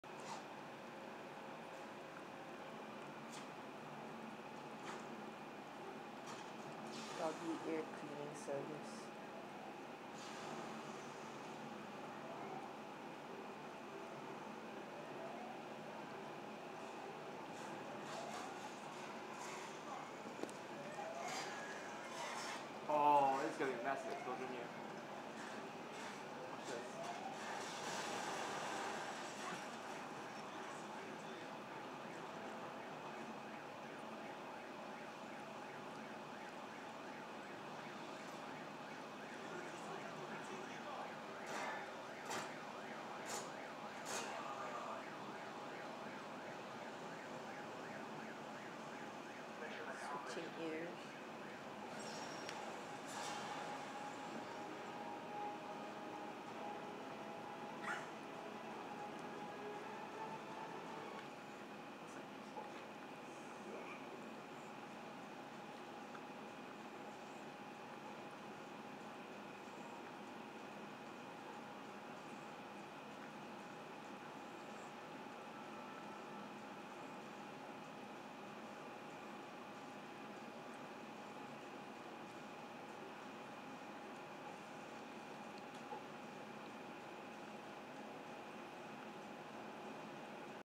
Halo Ear Cleaning Service
Dog ear cleaning service

Halo (shiba ) decided that Hobbes ( schnauzer ) ears were not up to the cleaniness standard